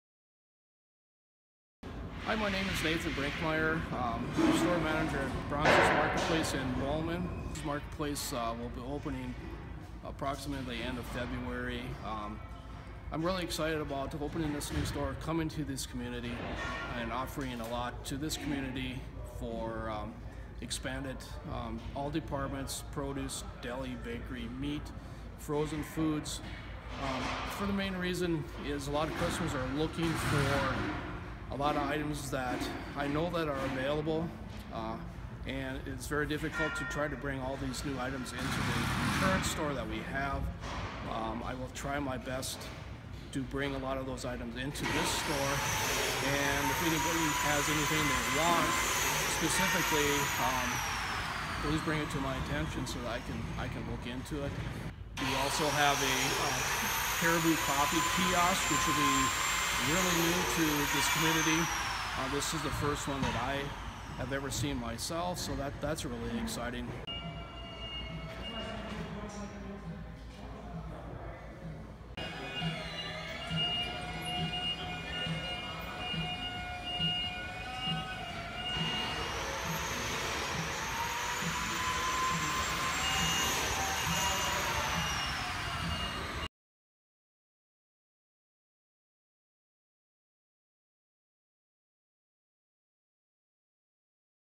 A look inside the new Bronson's Marketplace
The new location of Bronson's Marketplace in Bowman, N.D., is tentatively scheduled to open at the end of February. Watch as Store Manager Nathan Brinkmeyer takes The Pioneer inside the new store. (Video by Bryce Martin - Country Media, Inc. 2017)